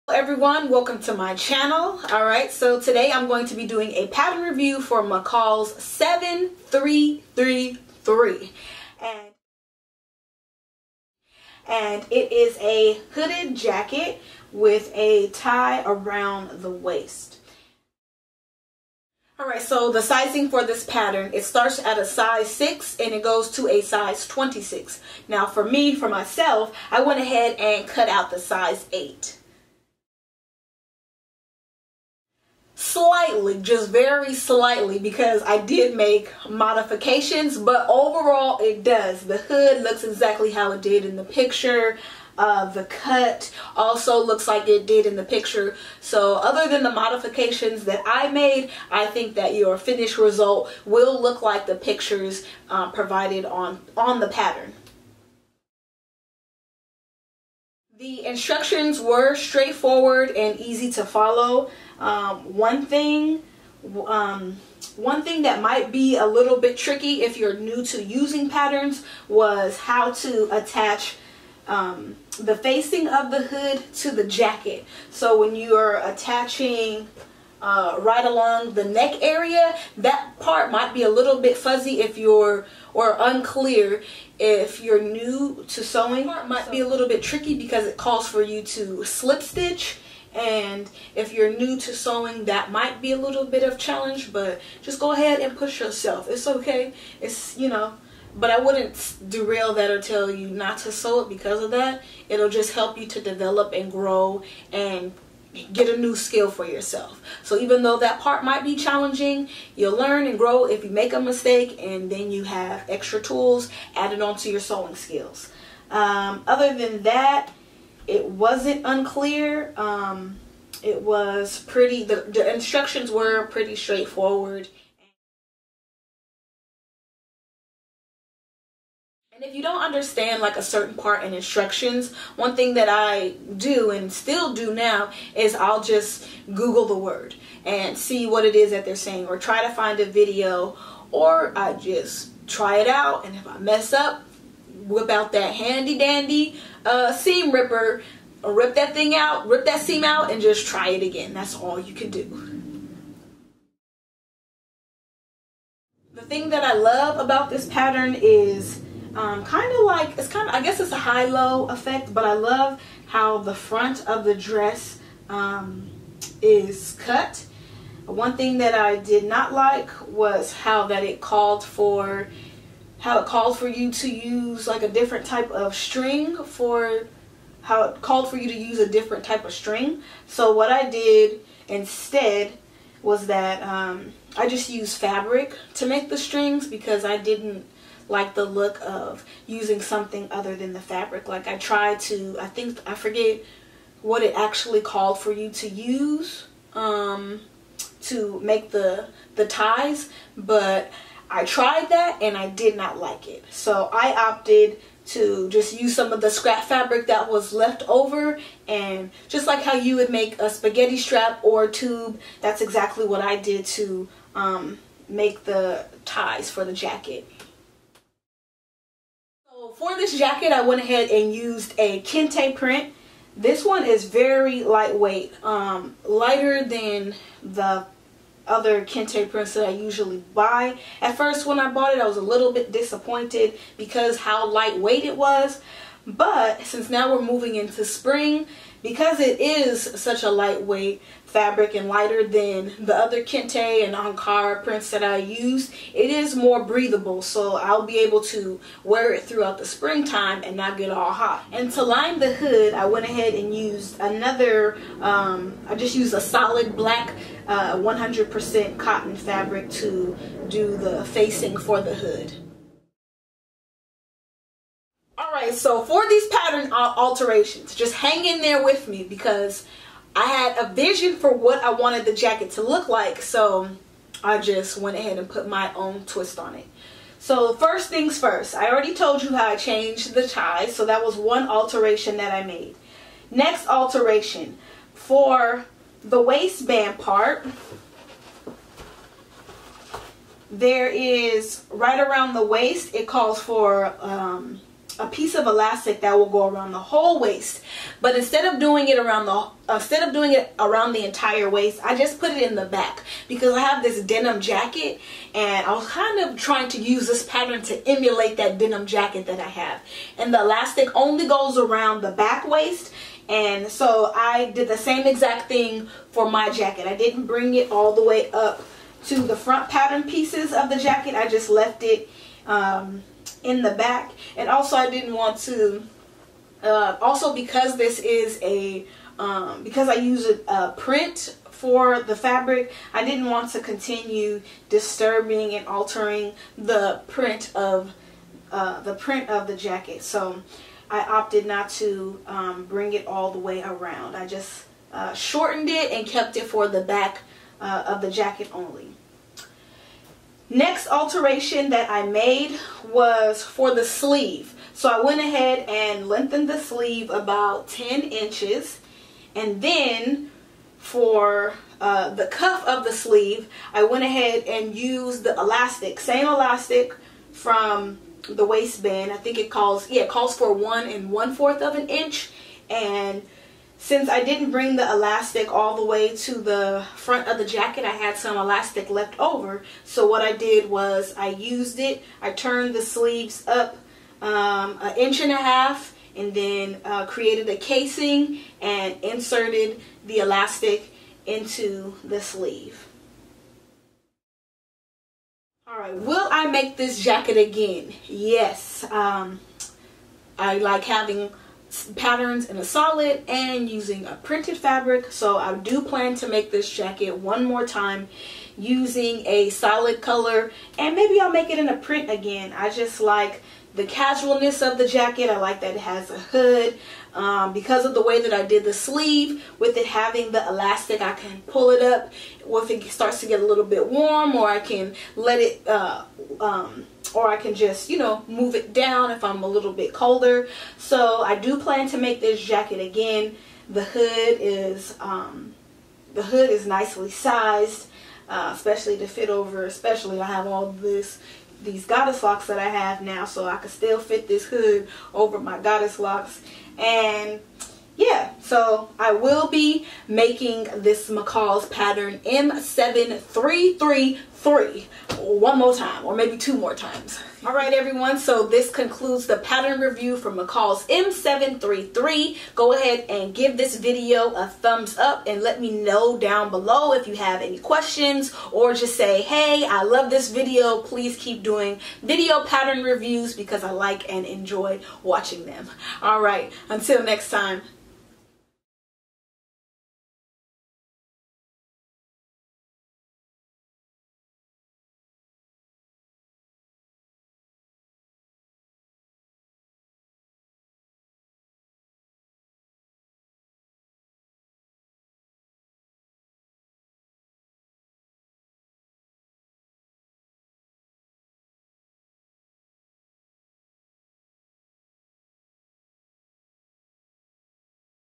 DIY Hooded Jacket: McCalls M7333 Review
Here’s a sewing pattern review of McCalls M7333 Hooded Jacket. I used kente print fabric to create this jacket.

Become a VIP and get access to free sewing resources

Upcoming Videos:
- Tube top Maxi Dress Tutorial
- Pattern Reviews